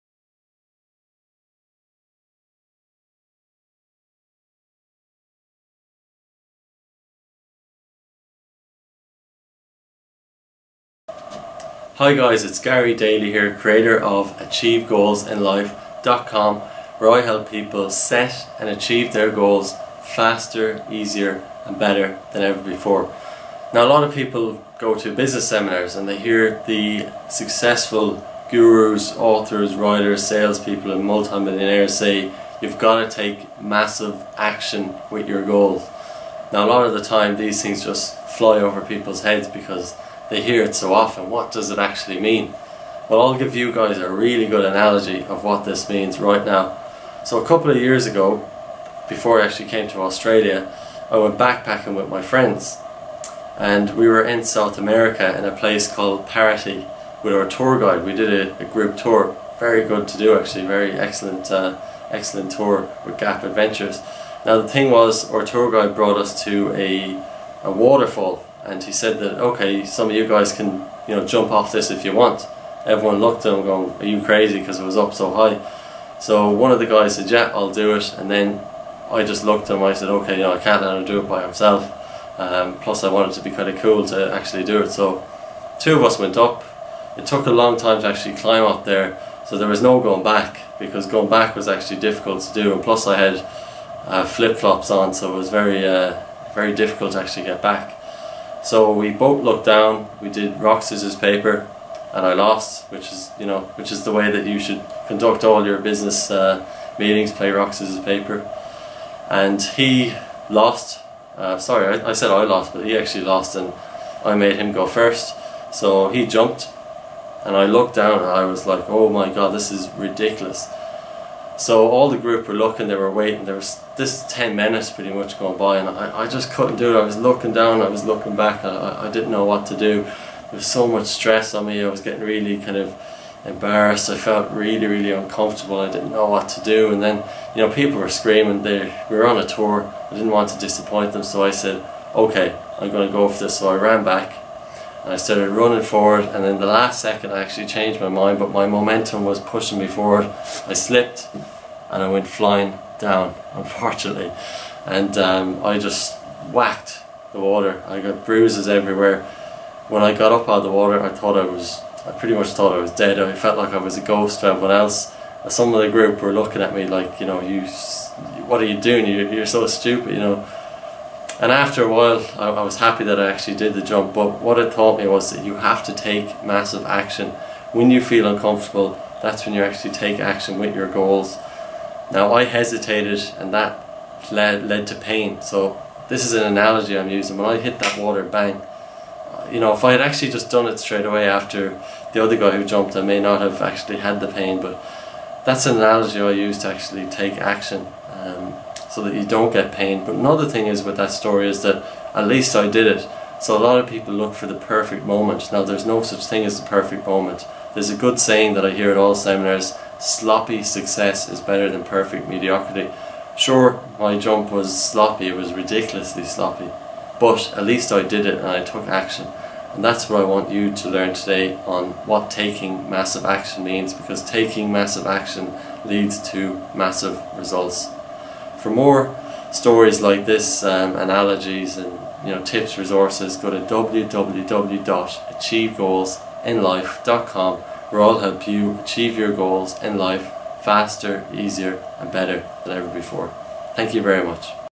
Learn How to Set and Achieve Goals by Taking Action!
I go to a lot of business seminars and they always give examples of goals from the top business gurus who consistently accomplish a goal. They know how to set and achieve goals by constantly taking action. Massive action leads to massive results! "Taking action" often flies over peoples heads because its become a bit of a cliche!!So what does it actually mean and why is it so powerful? Watch this video as I give an analogy of taking action for peoples personal goal setting so they understand the importance of taking actions with their goal setting. Let me know if you enjoy this below :) For more tips and resources go